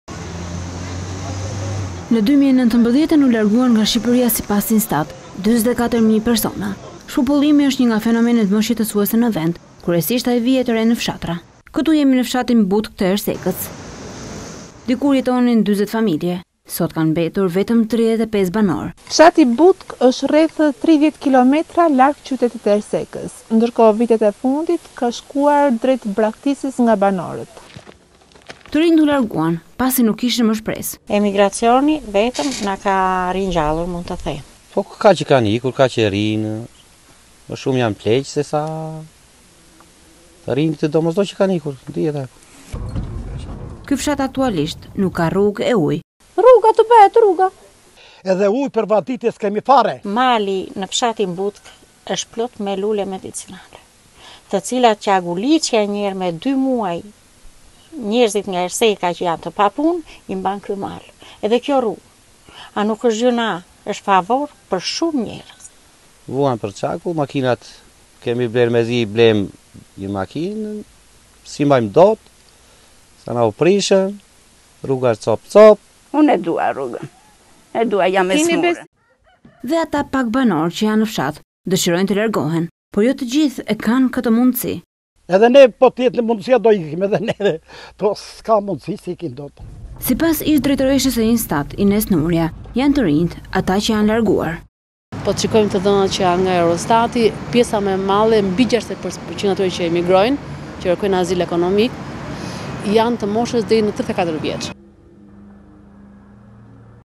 Fshati Butke me 35 banore kerkon ndertimin e rruges: Vite pa investime
Në 2019 u larguan nga Shqipëria sipas INSTAT 44 mijë persona. Shpopullimi është një nga fenomenet më shqetësuese në vend. Kryesisht ai vihet re në fshatra.

Këtu jemi në fshatin Butkë të Ersekës ku dikur jetonin 40 familje, sot kanë mbetur vetëm 35 banorë.

Fshati Butkë është rreth 30 km larg qytetit të Ersekës. Vitet e fundit ka shkuar drejt braktisjes nga banorët.

Të rinjtë u larguan pasi nuk kishin më shpresë.  Ky fshat aktualisht nuk ka nuk rrugë e ujë.

“Emigracioni vetëm na ka ringjallur. Toka bujqësore nuk prodhon”, u shpreh një qytetare.

Dhe ata pak banorë që janë në fshat dëshirojnë të largohen. Por jo të gjithë e kanë mundësinë.

“Do të ikja edhe unë. Po s’kam mundësi”, tha

Sipas ish-drejtoreshës së INSTAT, Ines Nurja kryesisht janë të rinjtë ata që braktisin vendin për një jetë më të mirë.

“Po të shikojmë të dhënat, mbi 65 % e atyre që emigrojnë janë të moshës deri në 34-vjeç”, tha ajo./abcnews.al/
Per informacionet me te fundit klikoni: